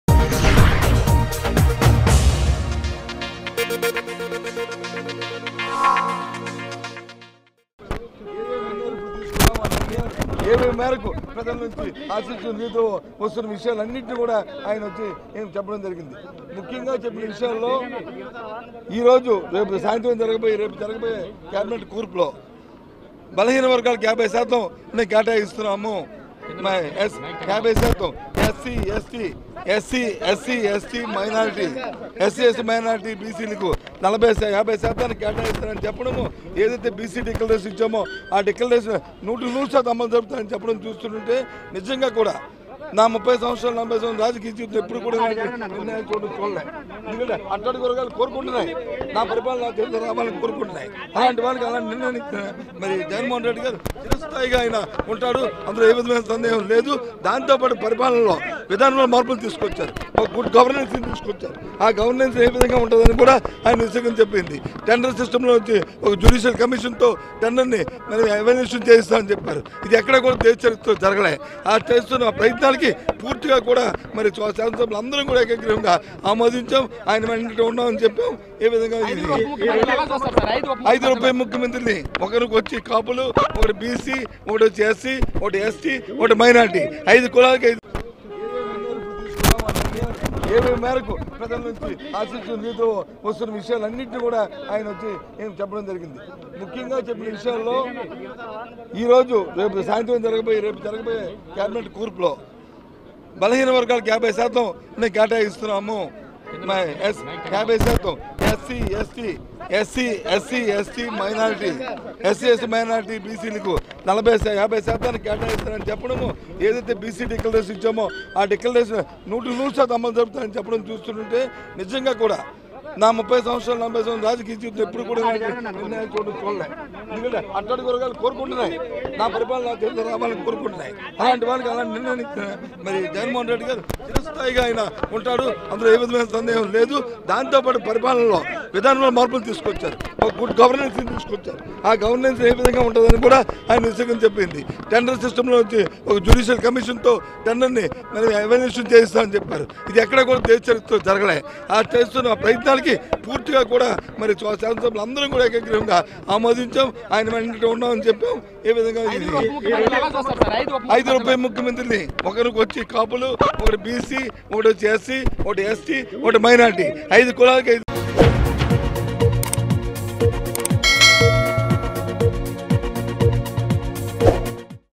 YSRCP MLA Bosta Satyanarayana Comments about YS Jagan Cabinet
Watch, YSRCP MLA Bosta Satyanarayana Comments about YS Jagan Cabinet